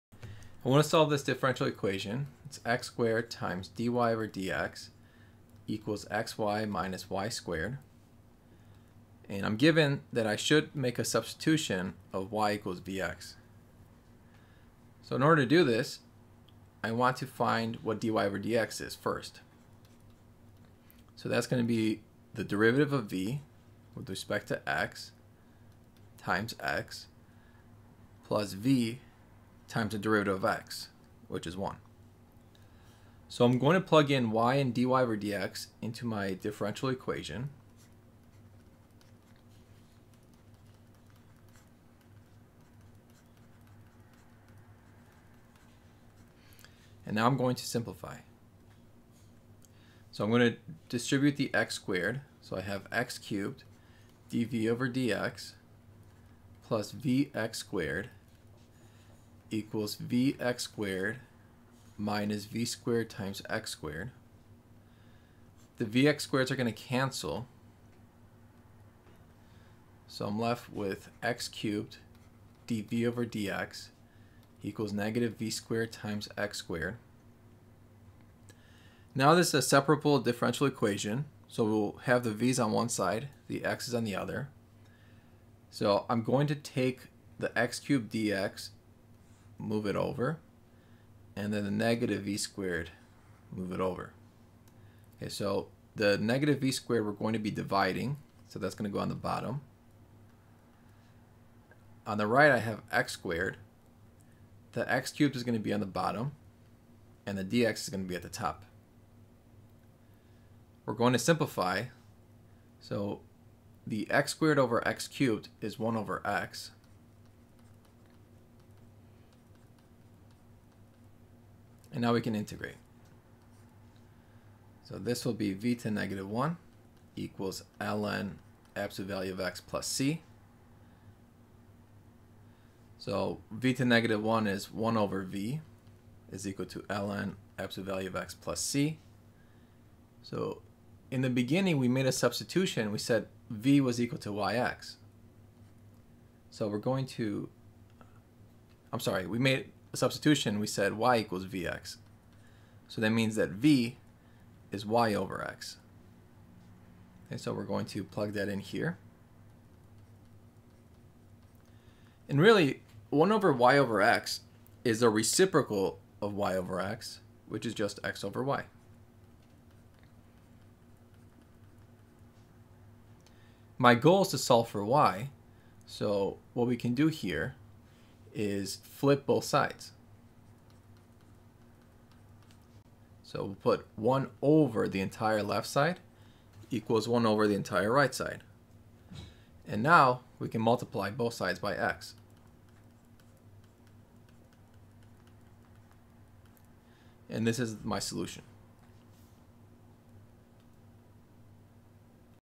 Diff Eq: x^2 dy/dx=xy-y^2
In this video we solve a differential equation x^2 dy/dx=xy-y^2 using y=vx substitution.